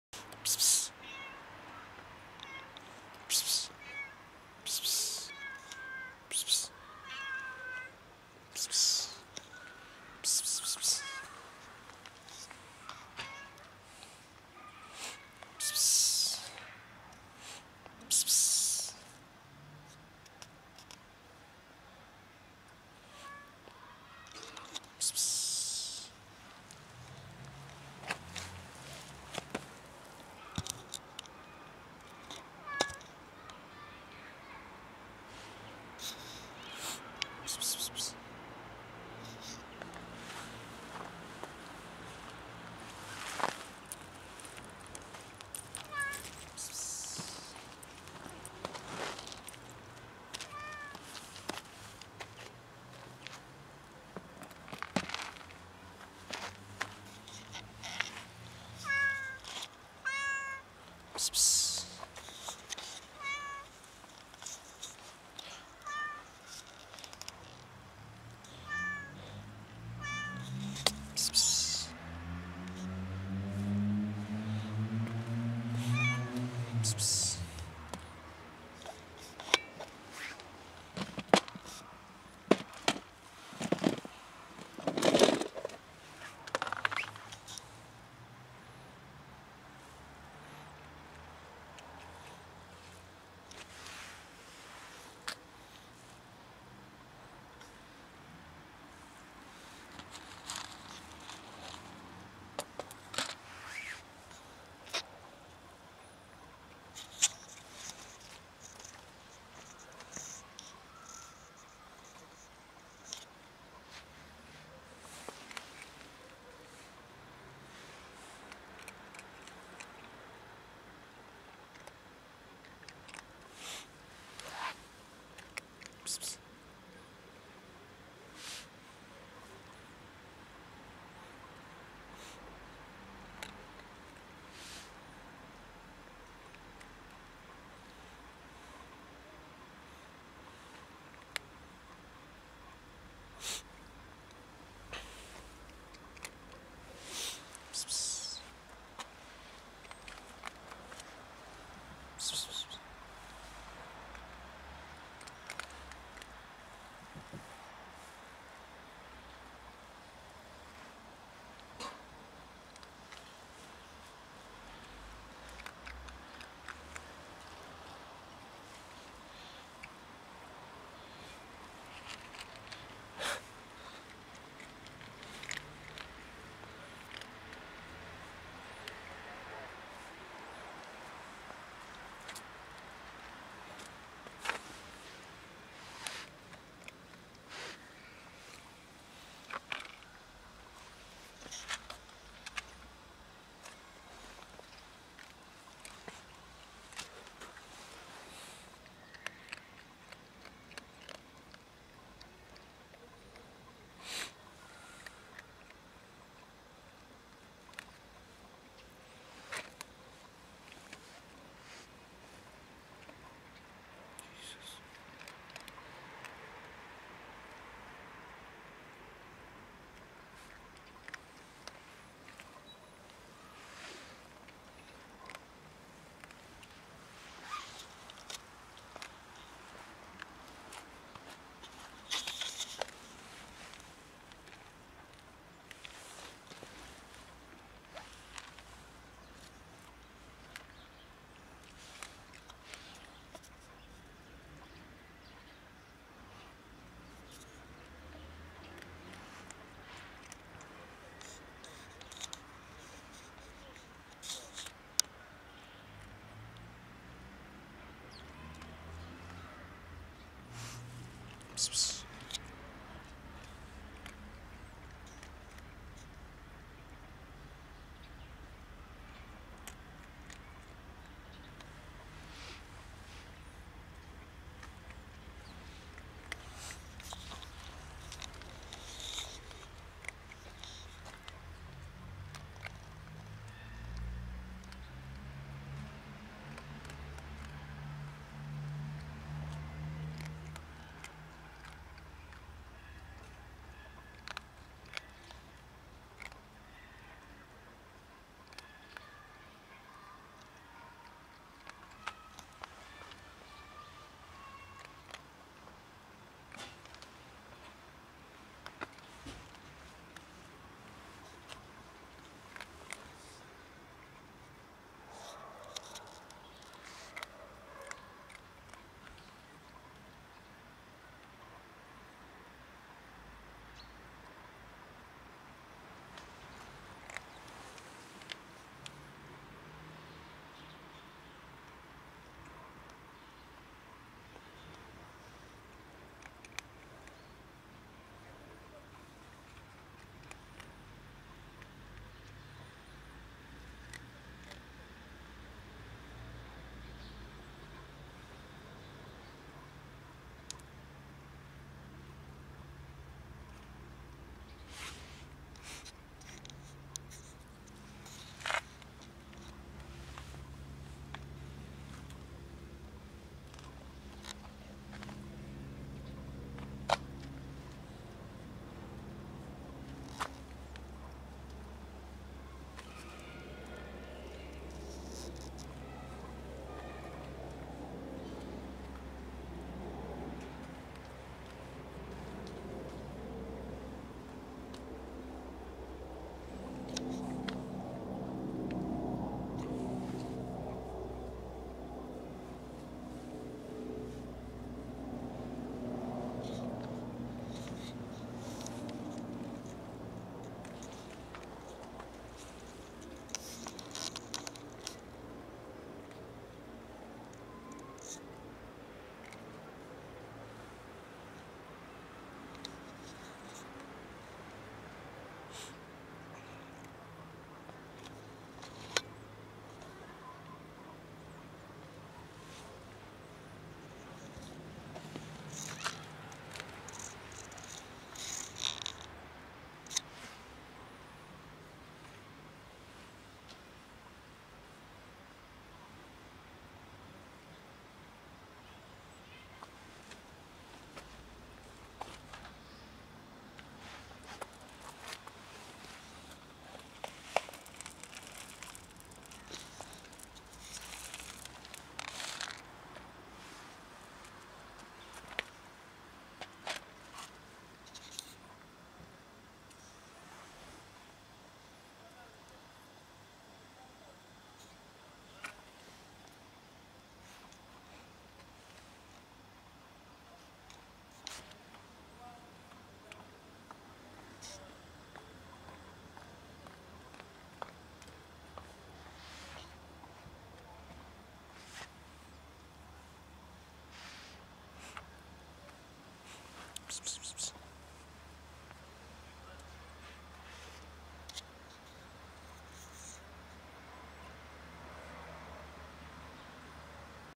Starving cat meows loudly asking for food - the cat was very skinny
My god, this cat was the skinniest I've ever seen. When I touched the cat, I could feel only skin and bones. Why is this happening? Is it so hard to feed the cats time to time, so this does not happen? Either way, I'll make this cat's surrondings my priority to visit regularly and possibly feed this cat on a regular basis.